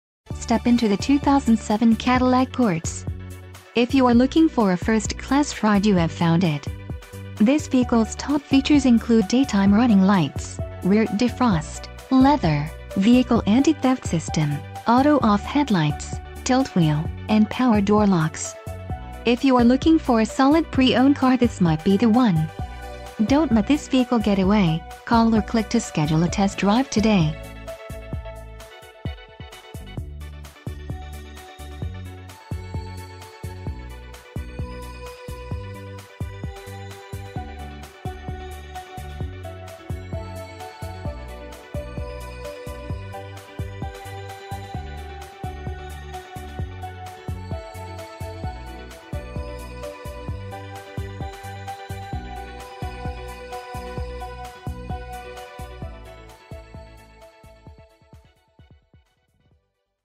2007 Cadillac CTS - Liberty GMC and Buick - Peoria, AZ 85
LOW MILES - 59,161! WHITE DIAMOND PREMIUM PAINT exterior and LIGHT GRAY/EBONY interior. Satellite Radio, CD Player, Onboard Communications System, Aluminum Wheels, Head Airbag, Heated Mirrors. 4 Star Driver Front Crash Rating. AND MORE!KEY FEATURES INCLUDEHeated Mirrors, Satellite Radio, CD Player, Onboard Communications System, Aluminum Wheels Keyless Entry, Remote Trunk Release, Child Safety Locks, Steering Wheel Controls, Traction Control. CTS with WHITE DIAMOND PREMIUM PAINT exterior and LIGHT GRAY/EBONY interior features a V6 Cylinder Engine with 210 HP at 6500 RPM*. EXPERTS CONCLUDEEdmunds Consumers' Most Wanted Sedan Under $35,000. Not only does the CTS boast sporty handling, responsive braking, and a tradition of technology innovation, its performance statistics allow it to compete with BMW, Mercedes, and Audi.  4 Star Driver Front Crash Rating. 4 Star Driver Side Crash Rating.